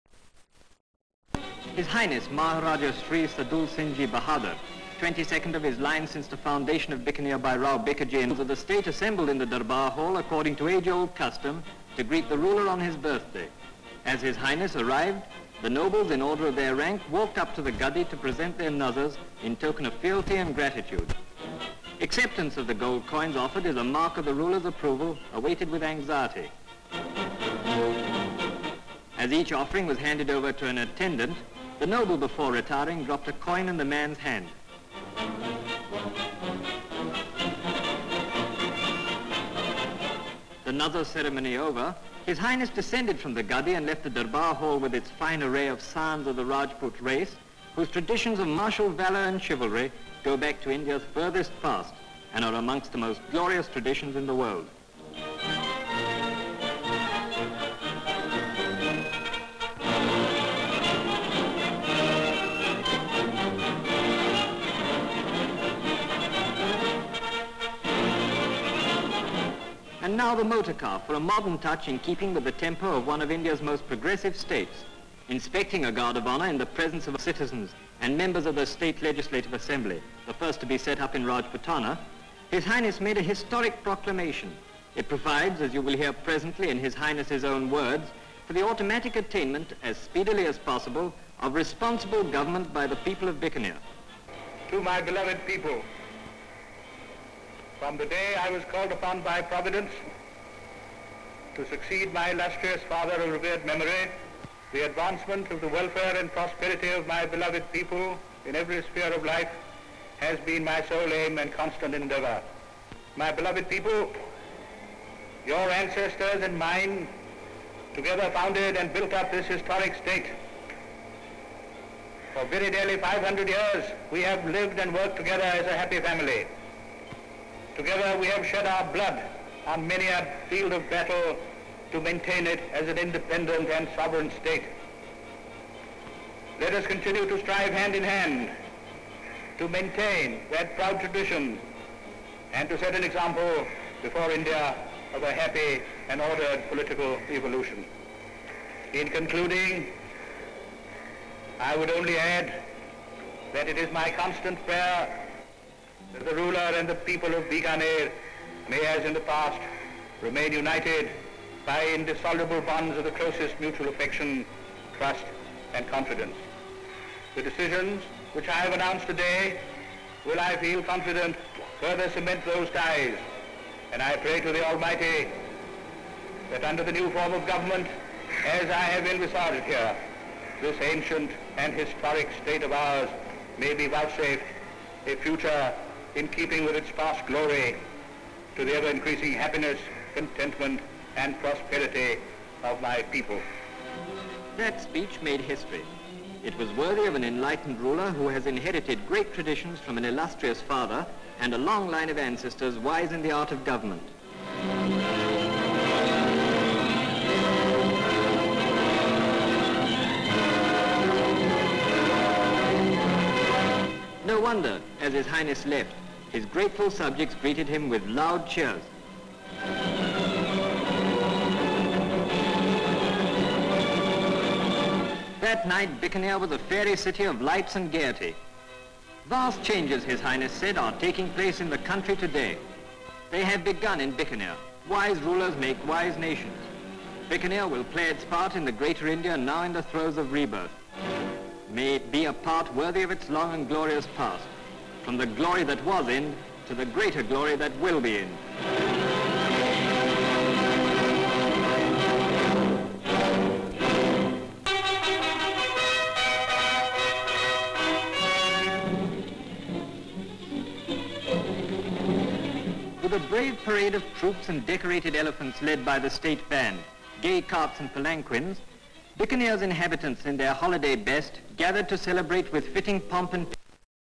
Maharajah Of Bikaner: A Royal Birthday (1946) | British Pathé
The Maharajah Of Bikaner India rides through the streets on his birthday to Durbar Hall. A slew of nobles line up to present him with an offering of a gold coin in this remarkable archive footage from 1946.

Maharajah of Bikanir.

M/S Various shots of the Maharajah riding through streets of Bikanir. Large crowd are gathered. C/U The Maharajah sitting cross legged in the Durbar Hall, whilst nobles, in order of rank walk up with their offerings. He leaves the Durbar Hall and is driven away in motor car. Shots of him inspecting guard of honour. He addresses the gathering. Various similar shots.